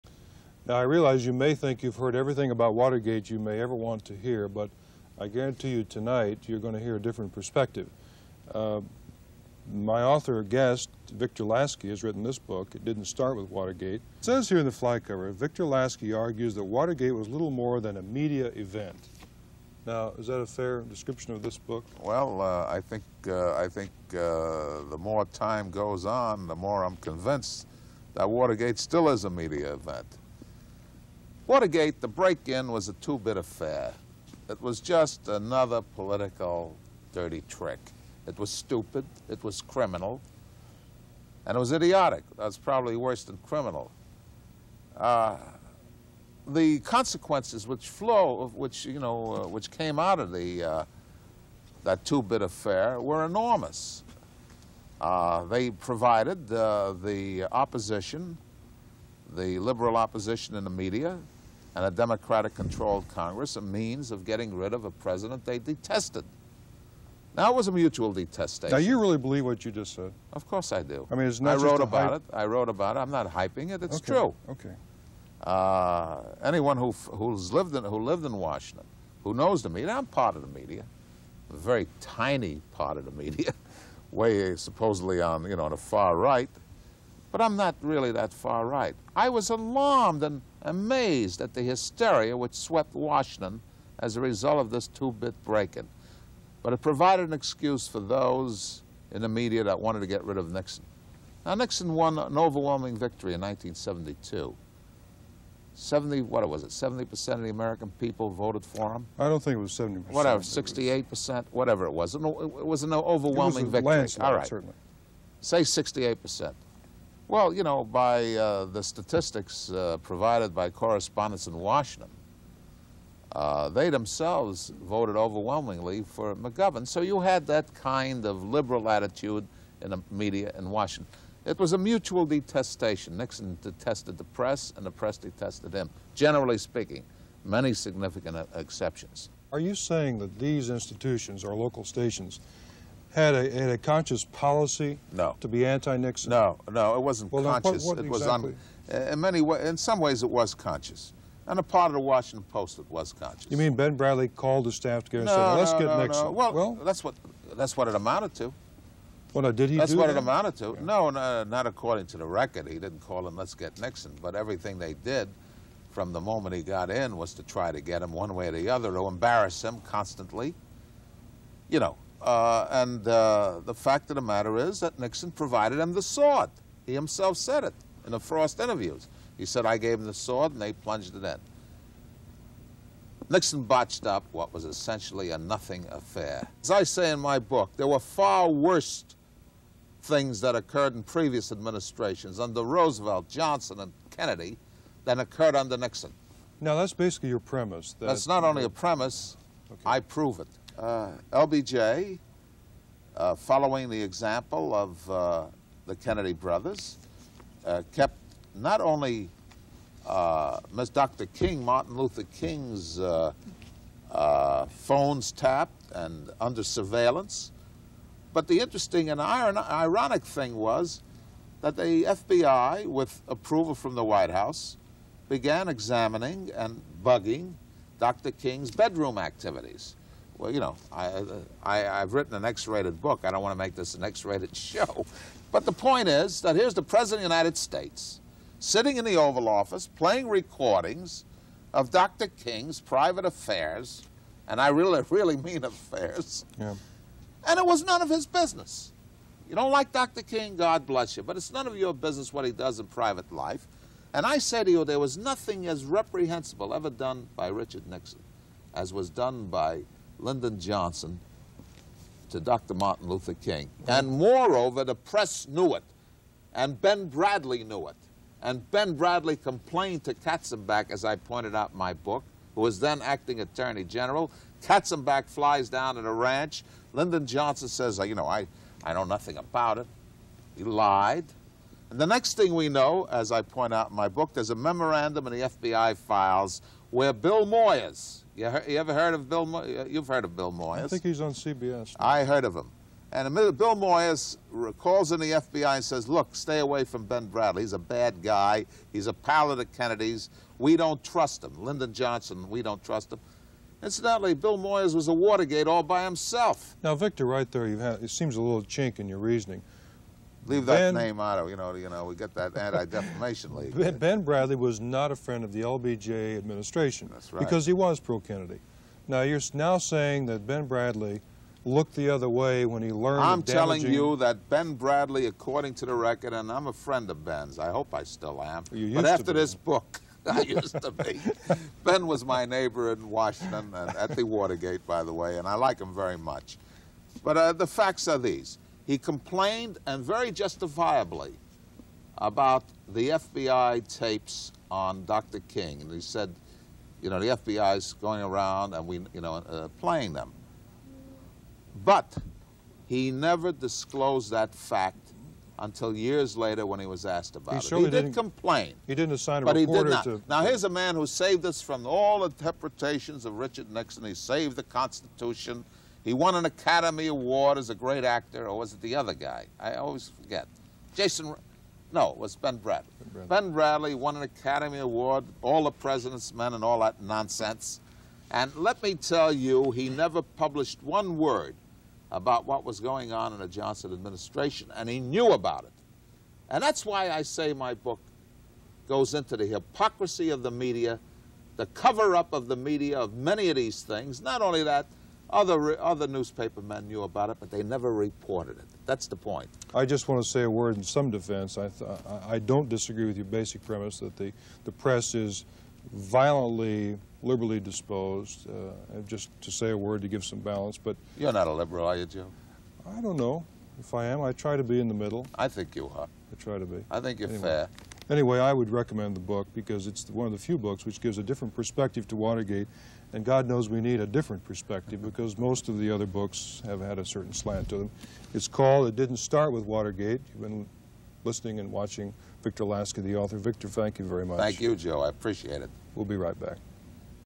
Victor Lasky Interview (August 9, 1977)
Victor Lasky (7 January 1918 – 22 February 1990) was a conservative columnist in the United States who wrote several best-selling books. He was syndicated by the North American Newspaper Alliance.

Background[edit]
On January 7, 1918, Victor Lasky was born in Liberty, New York. He graduated from Brooklyn College in 1940.

Career[edit]
In 1942, Lasky joined the U.S. Army and served during World War two; during that time, he did correspondence work for the army's newspaper Stars and Stripes.[2]

After World War Two, Lasky joined the staff of the New York World-Telegram; while there, he assisted Frederick Woltman in writing a series of articles on Communist Party infiltration within the US, for which Woltman won a Pulitzer Prize for Reporting in 1947.[2]

Lasky first came to prominence with his 1950 book Seeds of Treason, co-authored with Ralph de Toledano, in which the authors argued against Alger Hiss and in favor of Whittaker Chambers, with regard to Chambers' accusations both he and Hiss had been spies for the Soviet Union.

He was one of the first journalists to write a critical view of President John F. Kennedy. He expanded on this in his 1963 book JFK: The Man And The Myth, questioning Kennedy's wartime heroics on PT-109 and claimed he had a lackluster record as a congressman and senator. Lasky also wrote a similar negative book about Robert F. Kennedy.

Lasky's most controversial book was It Didn't Start With Watergate published in 1977. The author argued that the scandal that drove Richard Nixon from office was little more than a media event. He believed that the press disliked Nixon and subjected him to unfair scrutiny no other president had ever experienced. Lasky also claimed that Franklin D. Roosevelt, John F. Kennedy and Lyndon B. Johnson had used wiretaps on political opponents.

Lasky professed the greatest political "crime of the century" was not the Watergate scandal, but what he describes as the "theft" of the 1960 Presidential election.

In 1979, Lasky wrote another controversial work called Jimmy Carter: The Man And The Myth, asserting that Carter was one of the most inept presidents of all time.

Lasky's last work was Never Complain, Never Explain (1981), a biography of Henry Ford II.

Works[edit]
Books include:

1950 - Seeds of Treason; The True Story of the Hiss-Chambers Tragedy (with Ralph de Toledano)[3]
1960 - John F. Kennedy; What's Behind the Image?[4]
1963 - J. F. K.: the Man and the Myth[5]
1965 - The Ugly Russian[6]
1968 - Robert F. Kennedy; the Myth and the Man[7]
1970 - Arthur J. Goldberg, the Old and the New[8]
1970 - "Say ... Didn’t You Used to Be George Murphy?" (with George Murphy)[9]
1977 - It Didn’t Start With Watergate[10]
1979 - Jimmy Carter, the Man & the Myth[11]
1981 - Never Complain, Never Explain : the Story of Henry Ford II[12]
Articles include:

"How to Understand Communism," American Legion Magazine (August 1953)[13]